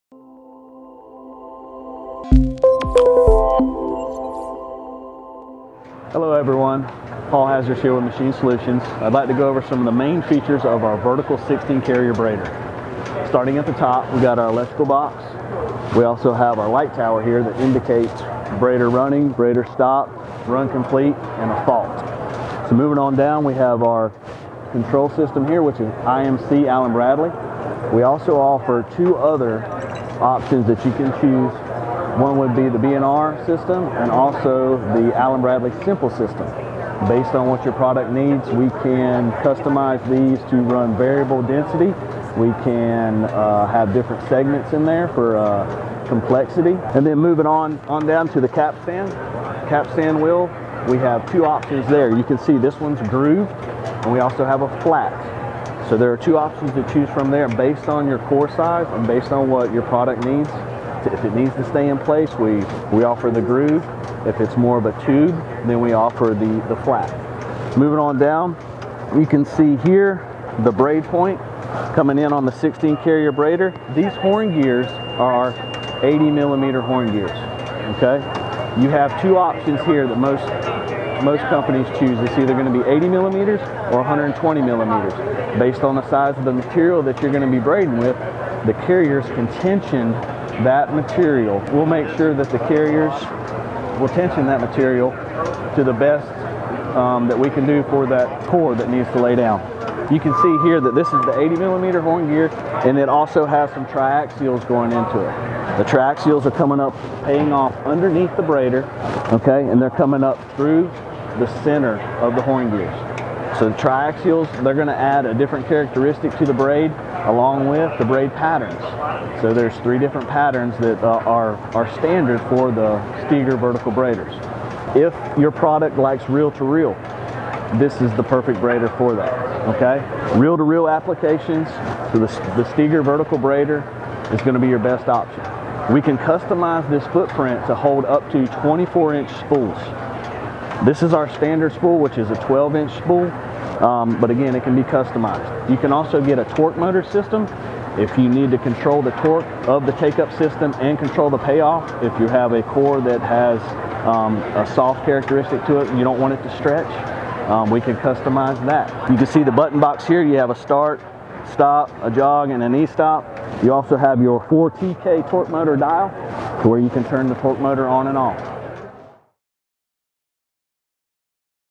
16 Carrier Braider Overview
Take a deeper dive into the main features of our Vertical 16 Carrier Braider with Paul Hazeres from Steeger USA. Learn more about the different configurations that are currently available and how we can provide the best braiding solutions for you.